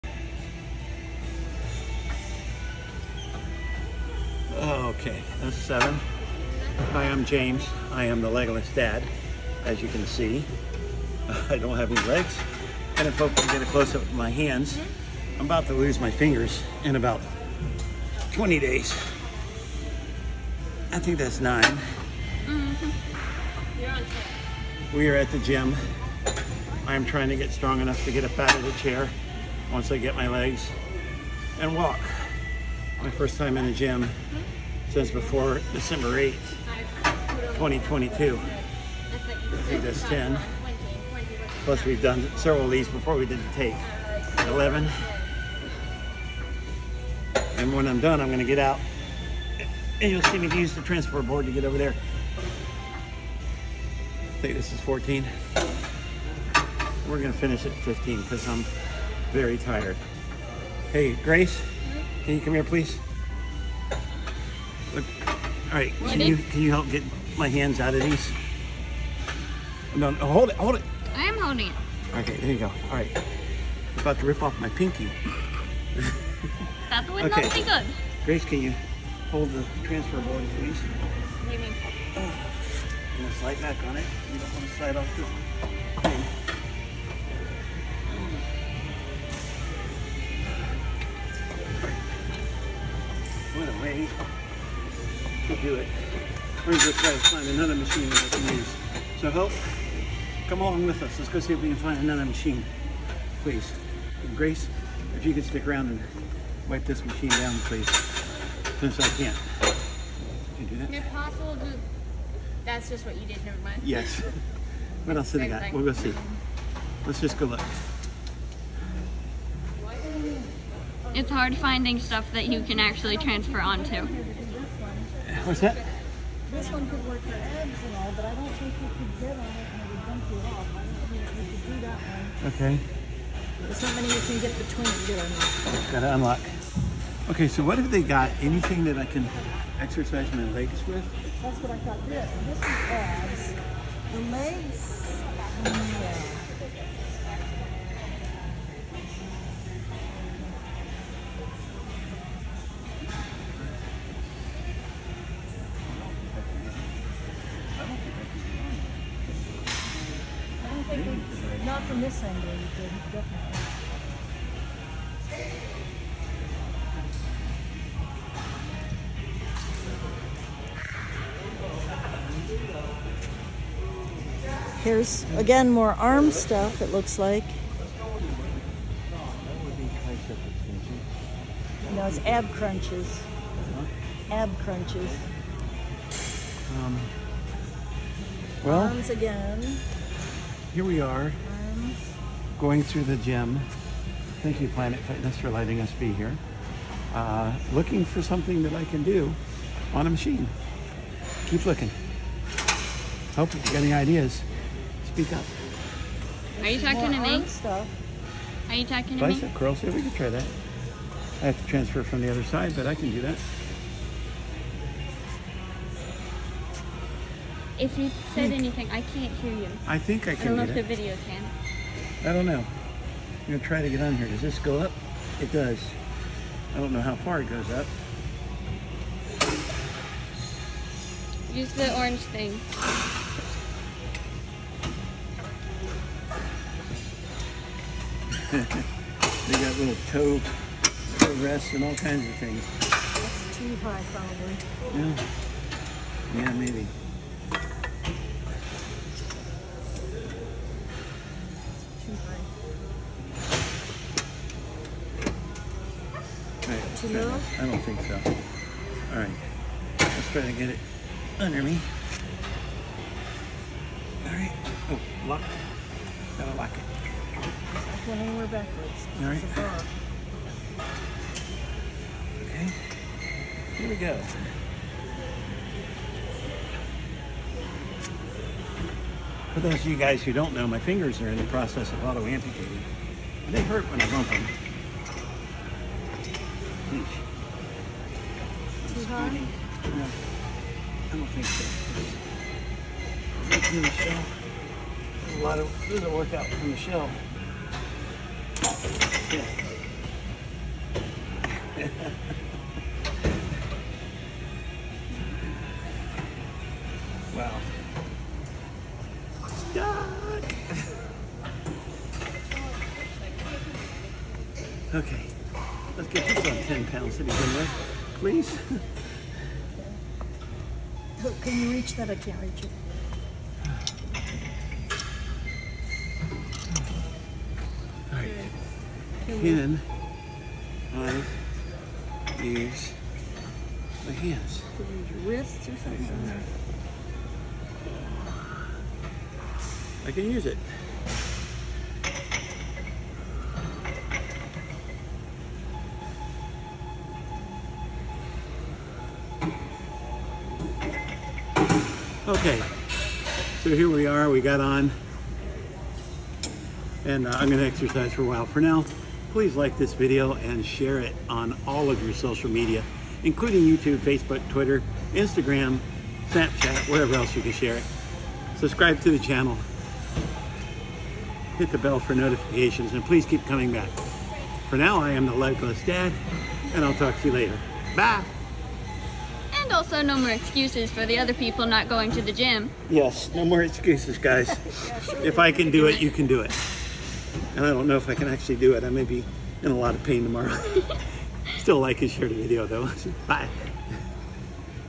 Exercising as an Amputee
Hi, I’m James, the legless dad. In just a few days, I will be getting my second set of test sockets and prosthetic legs. The first time I tried them on I could barely stand and I realized that I need to get much stronger.￼ The insurance company is telling me that physical therapy is not medically necessary so I joined Planet Fitness. Now, I will get stronger and be able to walk on my new legs.

This video is kind of long, but it shows how a bilateral below the knee and PT who is also unable to use his hands can get out of the wheelchair and onto the exercise machines and find a way to make it work. If you’re going through what I’m going through, be encouraged! You can do it too!!!￼